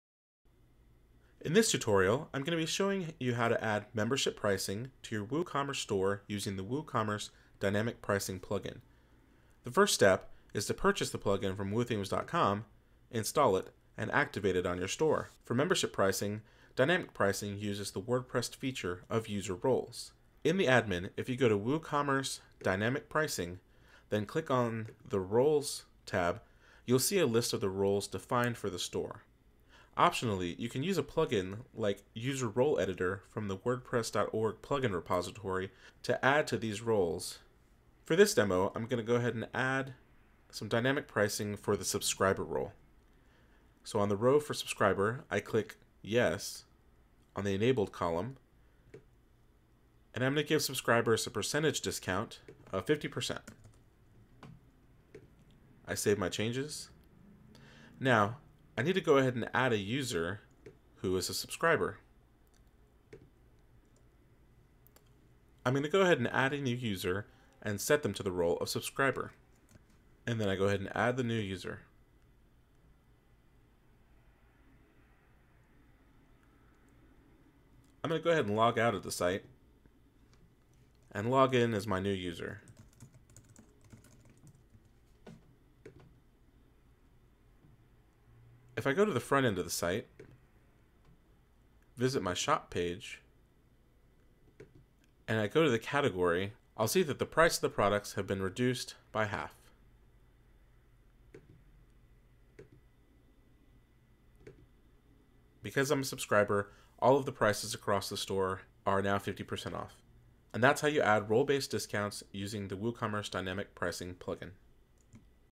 Role membership pricing using WooCommerce Dynamic Pricing
How to add a discount to user roles in WooCommerce using the WooCommerce Dynamic Pricing plugin.